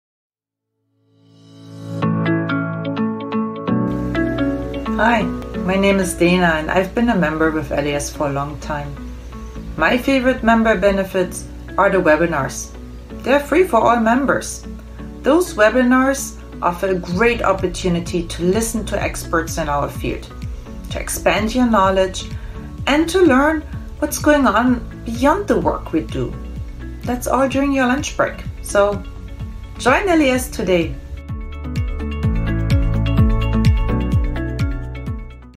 We are LES: Licensing Executives Society featuring Dana Matzen, Ph.D.
Meet Licensing Executives Society (LES) member, Dana Matzen, Ph.D., Vice President, Business & Corporate Development, Intel GenX Corp who shares her favorite LES member benefit!

When you join LES, you become part of a community of the best and brightest minds in the IP ecosystem. Expand your network and your knowledge, join LES today!